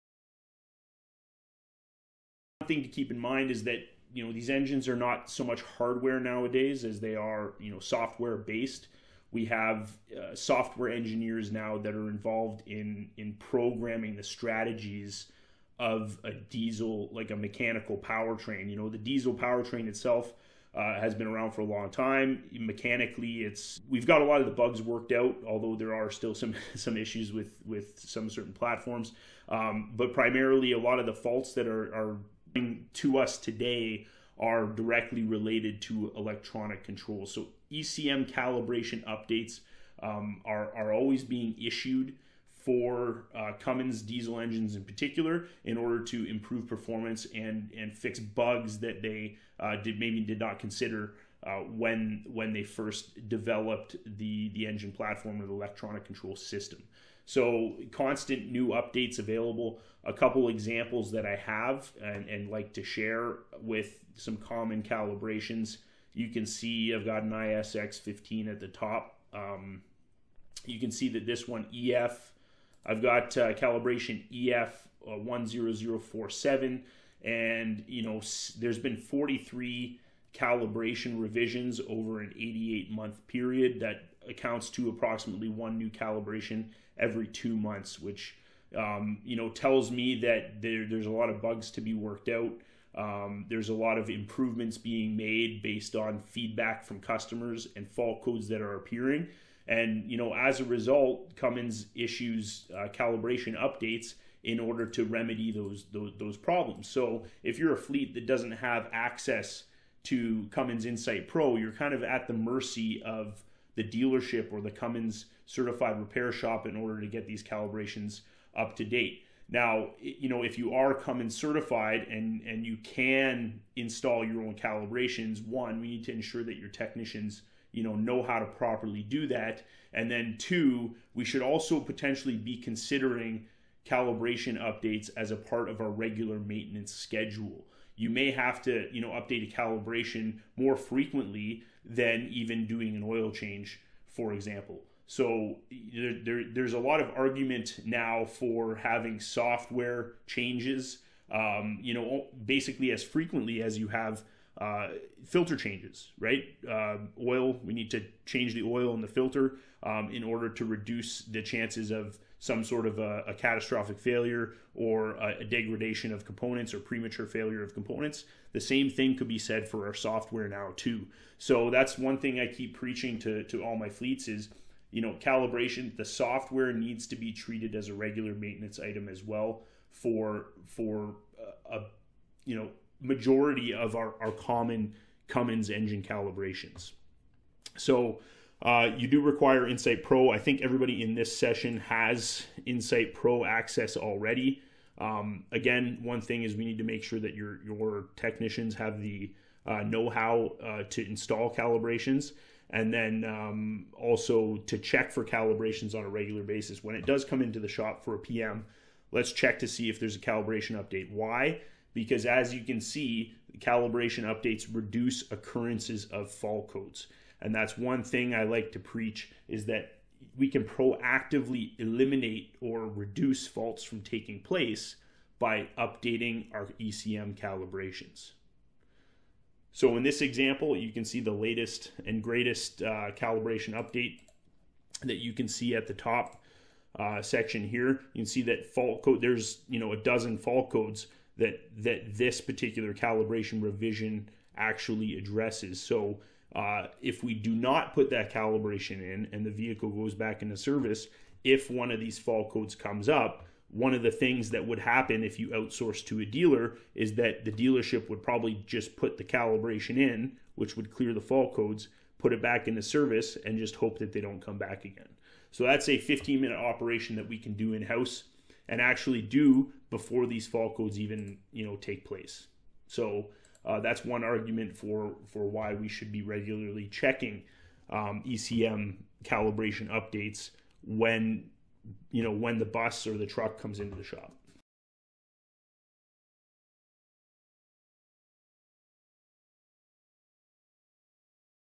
ECM Calibration Updates: What you need to know!
This excerpt from one of our live online training session explains the importance of frequent and regularly scheduled checks for ECM Calibration updates on modern diesel powertrains. The examples given are from ISX CM2250, ISX15 CM2350  and X15 CM2350 model Cummins engines.